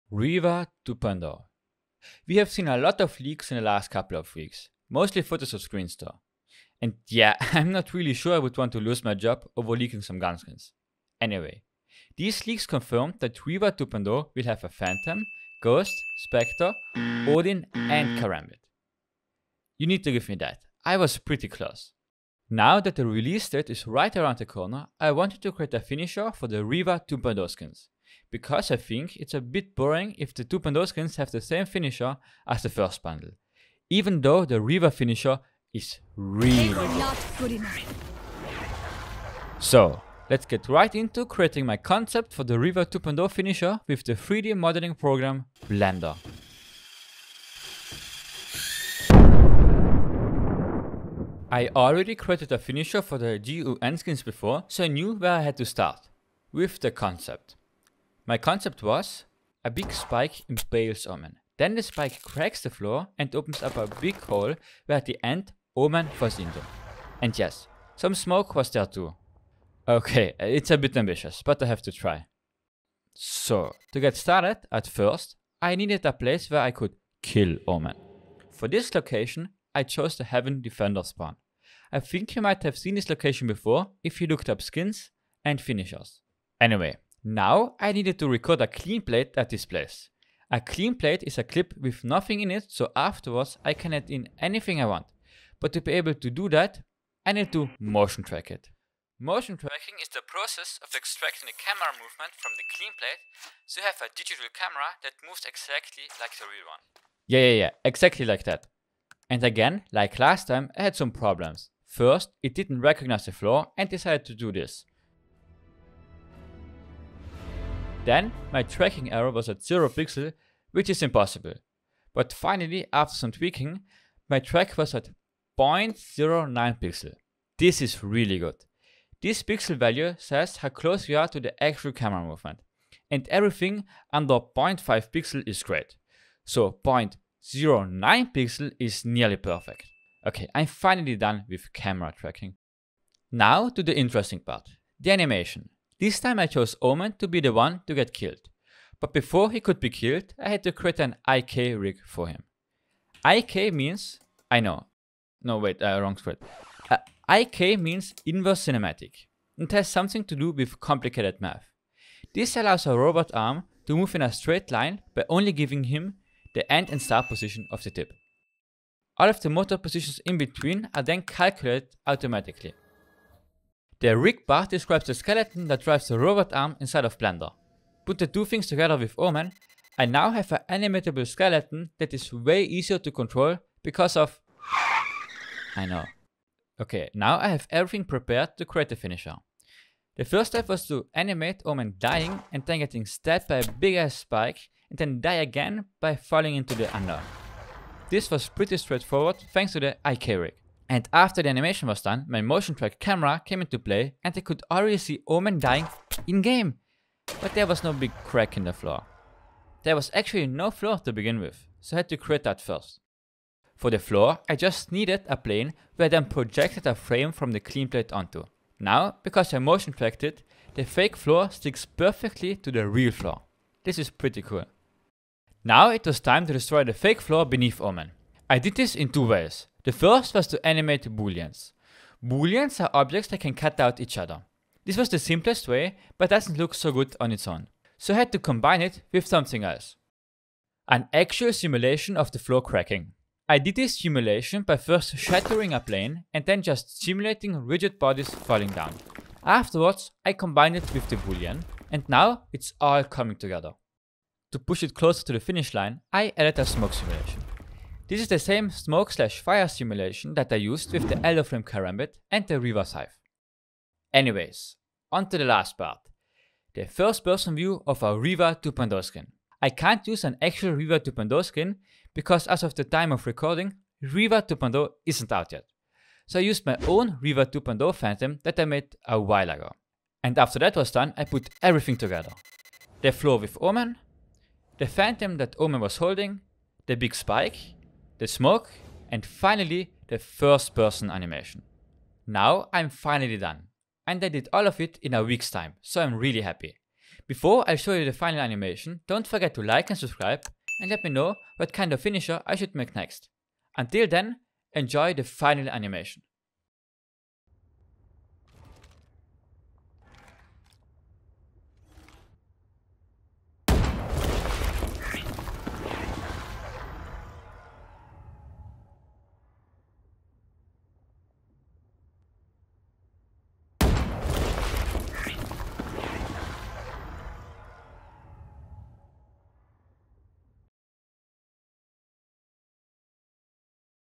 Reaver 2.0 Finisher Concept VALORANT | Blender 3.2
This week Thyros made a Reaver 2.0 Finisher and the released it together with eh actual Reaver 2.0 Skins, like the cool Reaver Phantom.

Thx for watching and take care :)